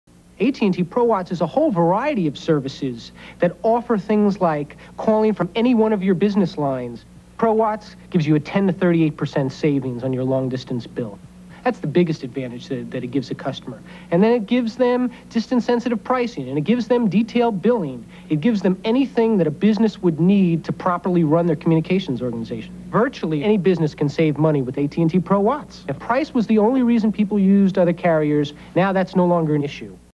AT&T - Pro Wats - Commercial - Business Long Distance Phone (1989)
Transferred from VHS Recording of "Smithsonian Treasures" on KTZZ Channel 22 in Seattle. Recording was made in early April 1989